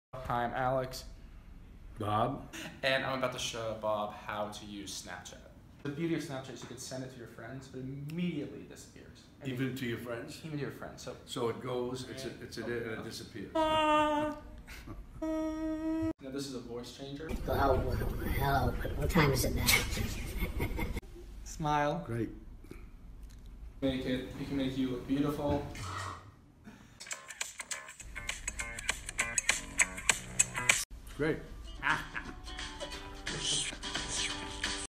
Robert De Niro and Alex Berry Review Snapchat at the 2017 Tribeca Film Festival
Alex Berry is a filmmaker and was a finalist in the 2017 Tribeca Snapchat Shorts. Berry shared insight on Snapchat's features and filters with Robert De Niro, co-founder of the Tribeca Film Festival.

Video provided by: Tribeca Film Festival